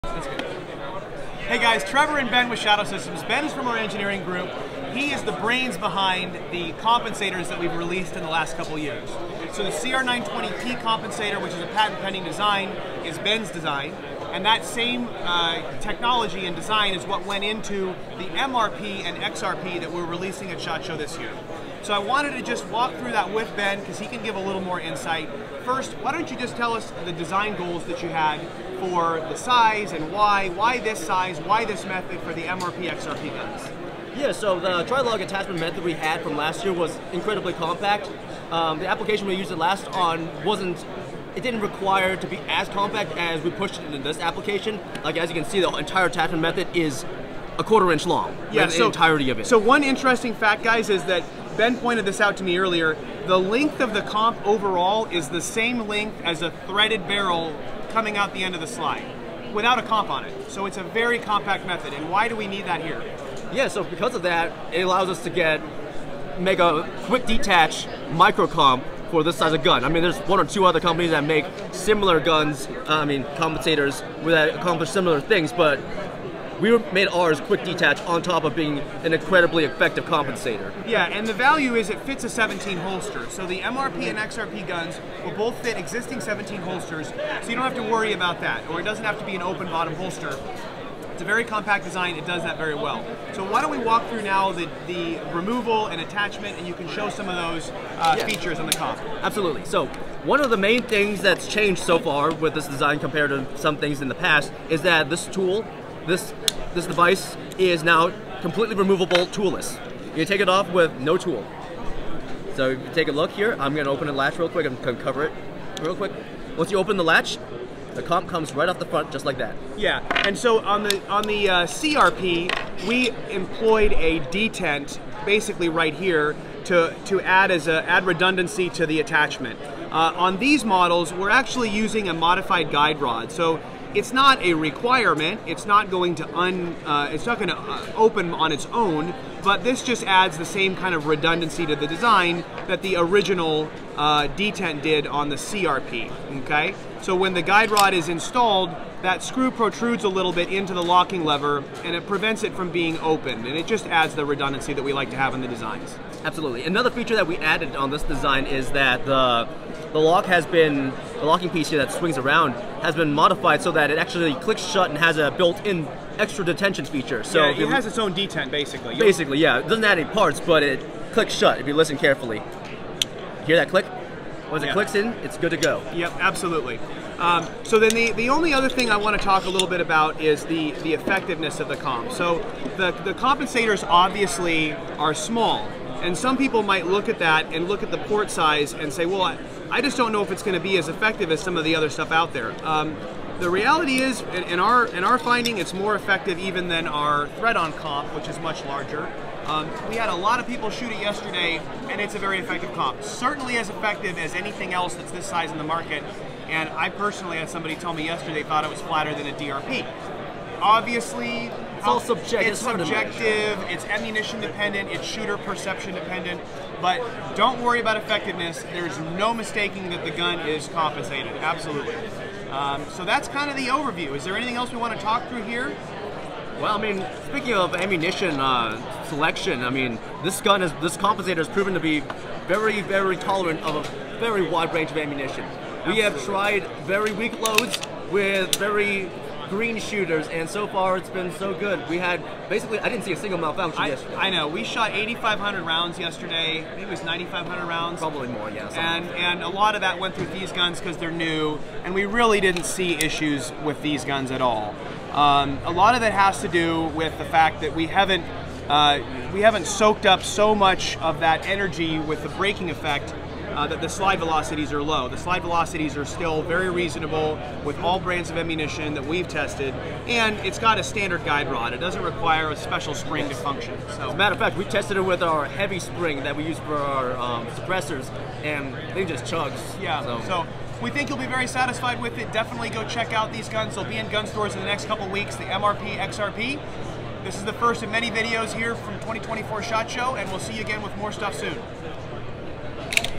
SHOT Show 2024: Getting into the Engineering behind the NEW MR920P and XR920P pistols
Trevor and Ben get into all of the engineering that went into the newly released MR920P and XR920P compensated pistols. MR920P and XR920P both use the tri-lug quick detach method you may be familiar with from our CR920P.

XR920P and MR920P both fit G17 holsters. The length of the built in compensator on these pistols is the same length as a threaded barrel on the non-comped versions of the MR920 or XR920, making this one of the smallest 9mm compensators in the market with quick detach functionality.

The compensator attachment method for MR920P and XR920P is now TOOL-LESS!